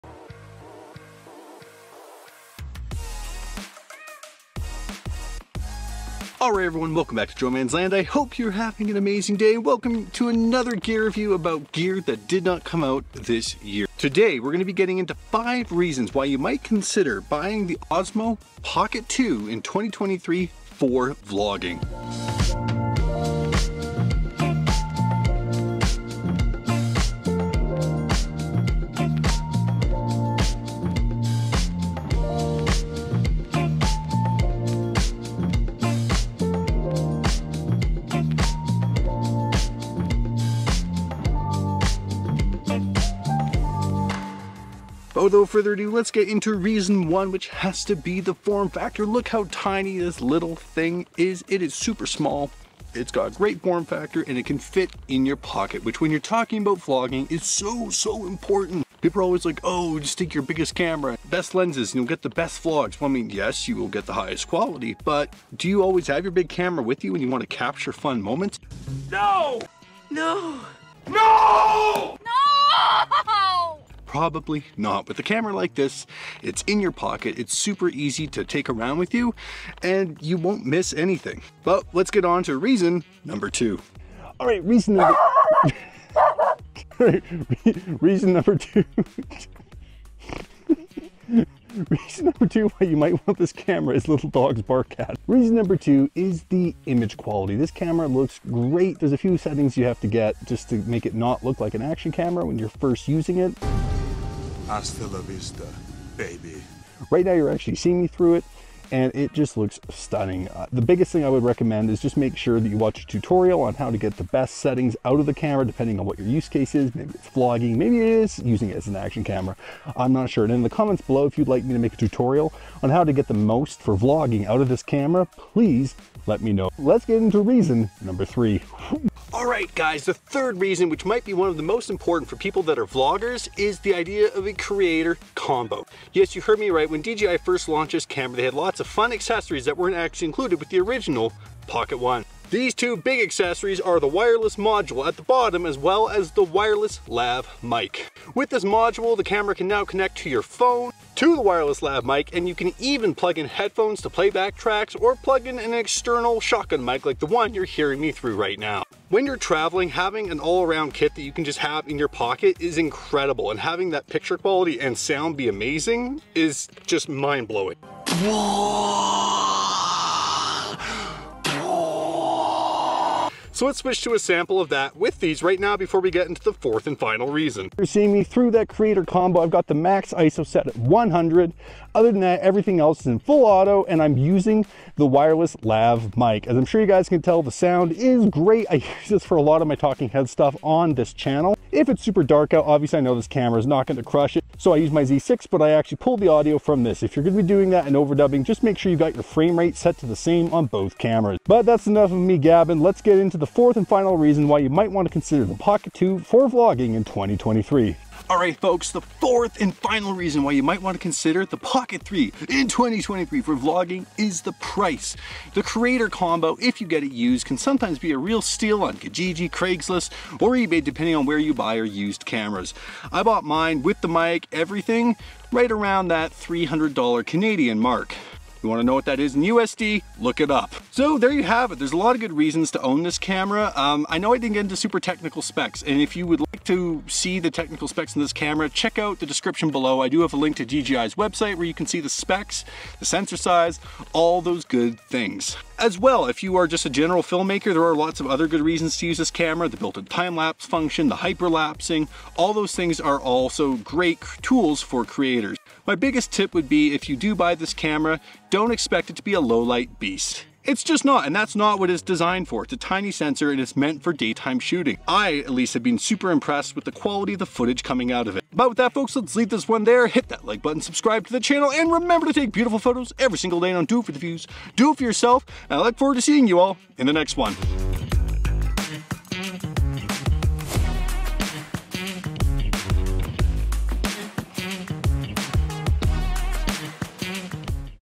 Pocket 2 in 2023 - ULTIMATE Vlogging Camera
As 2023 continues to role on, some of you might be wondering what you should use for vlogging as more and more cameras are released. My go-to is the Pocket 2. For the price it packs a punch and can deliver killer footage!

What are your thoughts on the Pocket 2?

My Site:
jodigital.ca